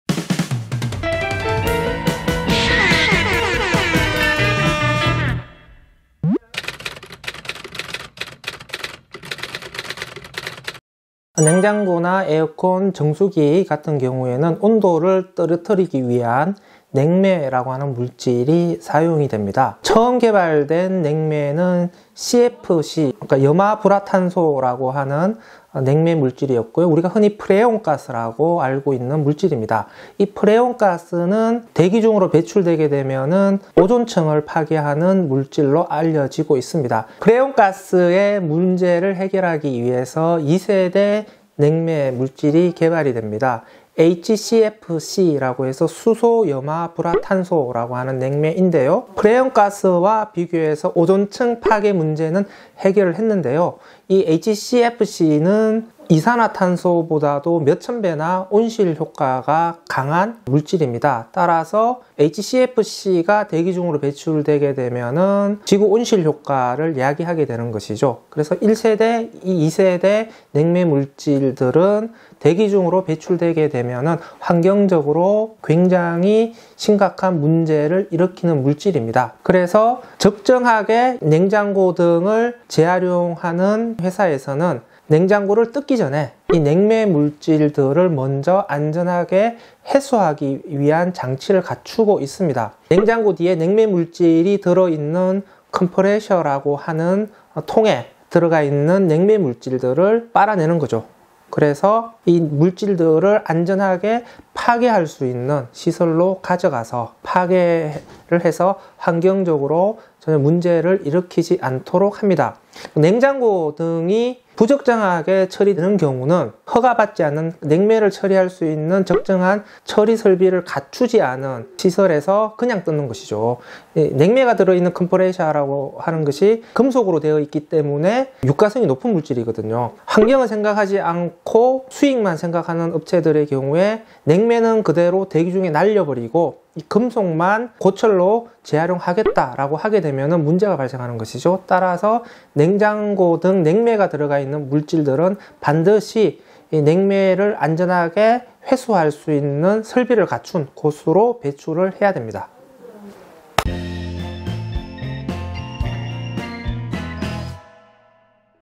냉장고는 버려진 후 어떻게 되나요? | 도와줘요 쓰레기박사 | 홍수열 자원순환사회경제연구소장
"냉장고나 에어컨 등 냉매가 들어있는 전자제품은 버려진 후 어떻게 되나요?"

플라스틱·자원순환·쓰레기, 무엇이든 물어보세요!
자타공인 쓰레기박사, 홍수열 자원순환사회경제연구소 소장님이 답해드립니다.

속 시원~히 알려주었으면 하는 질문들, 댓글로 남겨주세요 :)

✔︎ 이 영상은 한국전자제품자원순환공제조합과 함께 제작하였습니다.

▶ 더 유용한 영상을 위한 문자후원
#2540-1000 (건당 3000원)